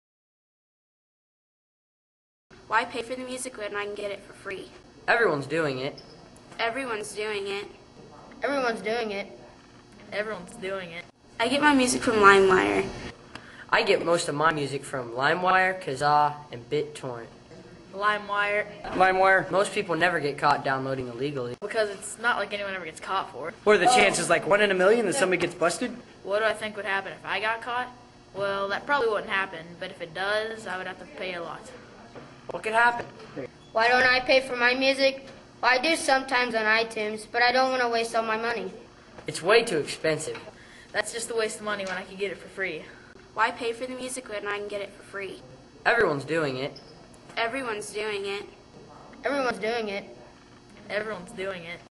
Interviews
Kids being interviewed about Limewire